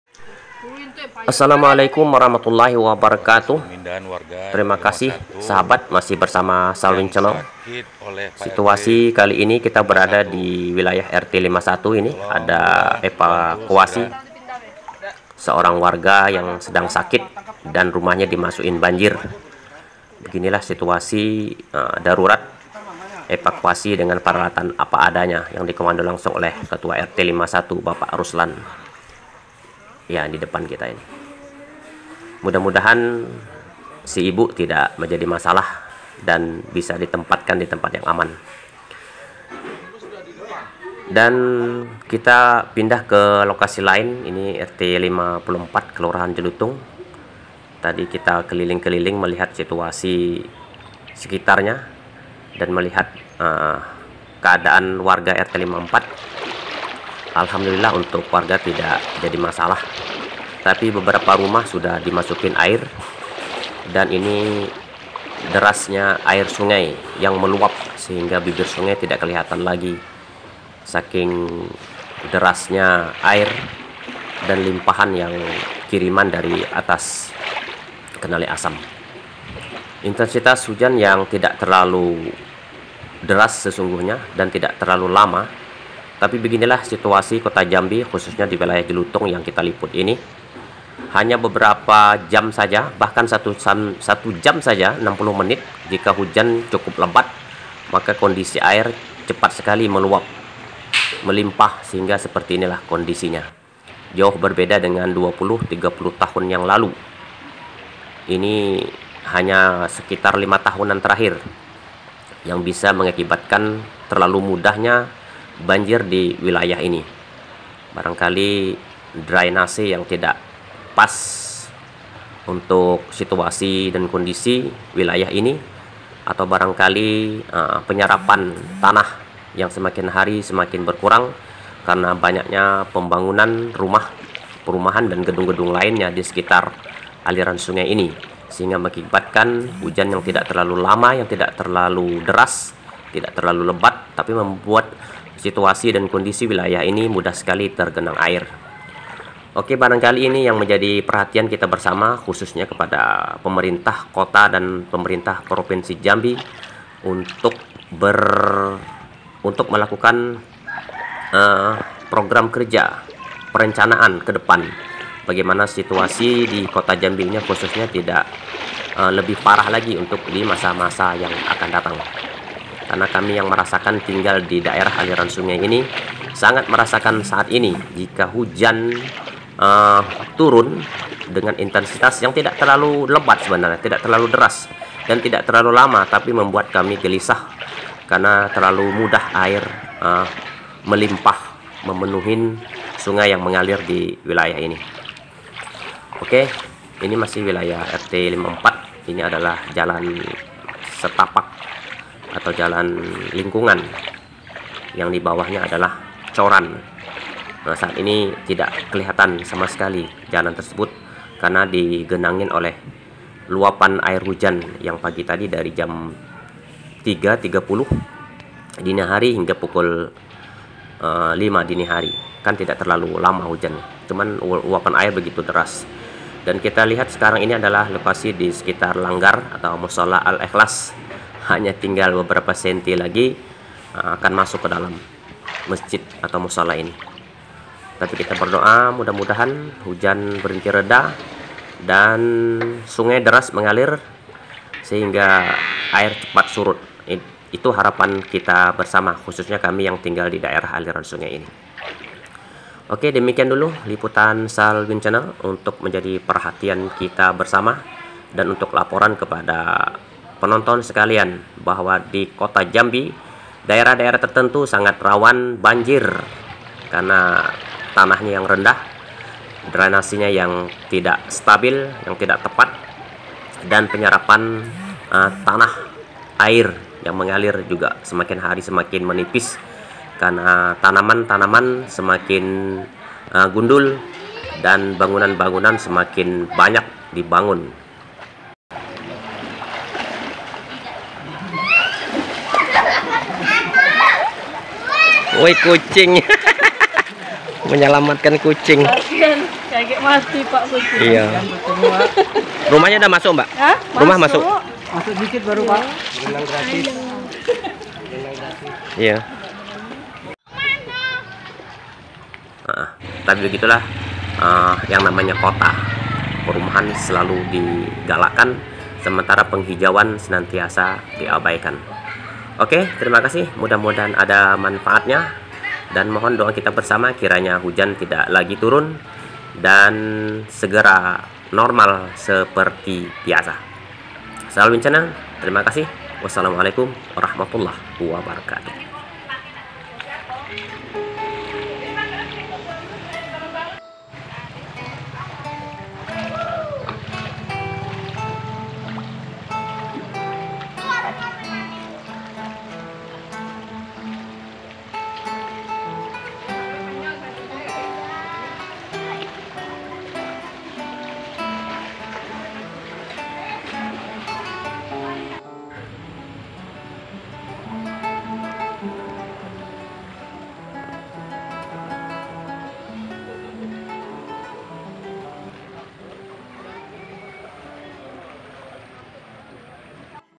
EVAKUASI WARGA DENGAN PERALATAN APA ADANYA. KOTA JAMBI DILANDA BANJIR
Intensitas hujan yang tidak terlalu tinggi, namun membuat beberapa wilayah Kelurahan Jelutung Kecamatan Jelutung Kota Jambi digenangi air hingga masuk ke rumah warga